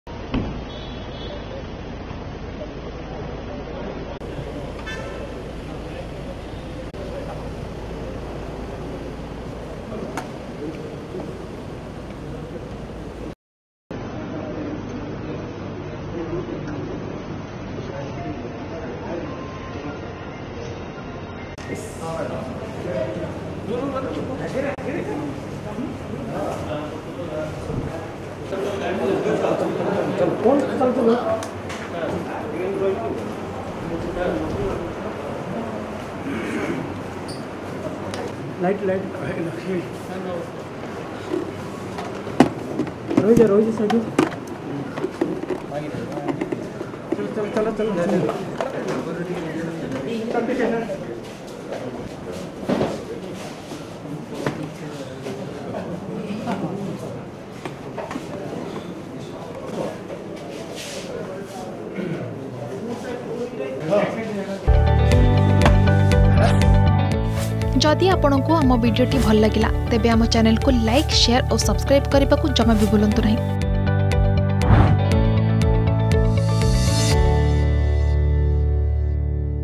ଲୋକସଭା ଭବନରେ ଆଜି ଆଇନ ମନ୍ତ୍ରୀ ପୃଥୀରାଜ ହରିଚନ୍ଦନ ନିଜ ବିଭାଗର ଦାୟିତ୍ୱ ନେଇଛନ୍ତି || BJP Govt
ଲୋକସଭା ଭବନରେ ଆଜି ଆଇନ ମନ୍ତ୍ରୀ ପୃଥୀରାଜ ହରିଚନ୍ଦନ ନିଜ ବିଭାଗର ଦାୟିତ୍ୱ ନେଇଛନ୍ତି ।